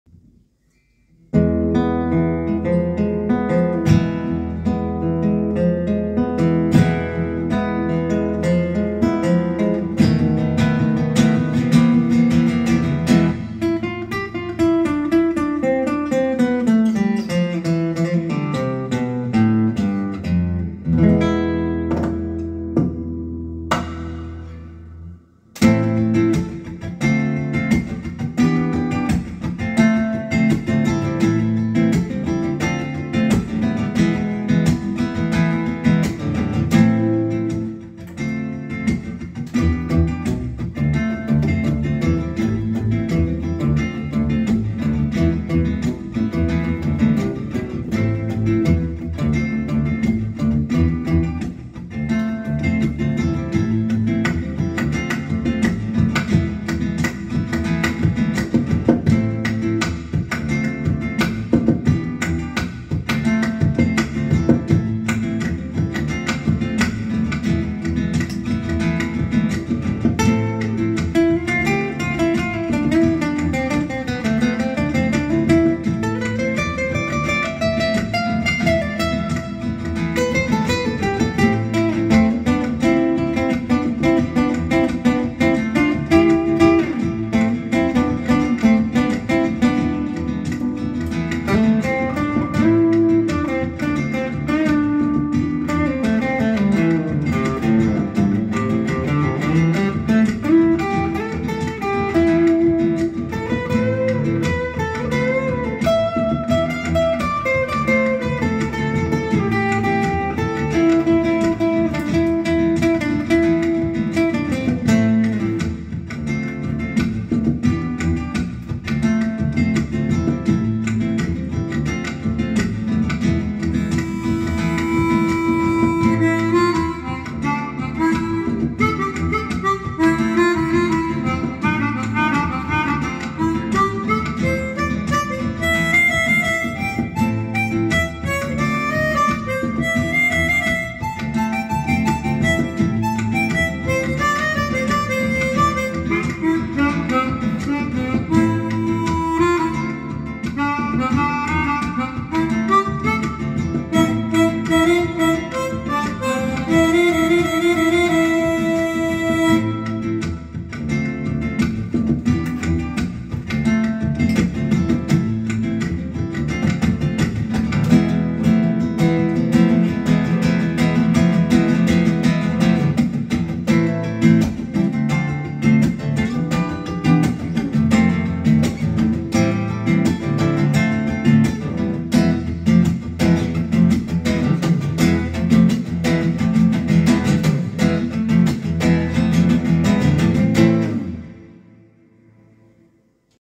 Latin Style looping - Boss RC-500
Trying to relive the summer in these winter months, with some Latin style looping

Check out the links to my looping and guitar lessons below.

Two chord Reggae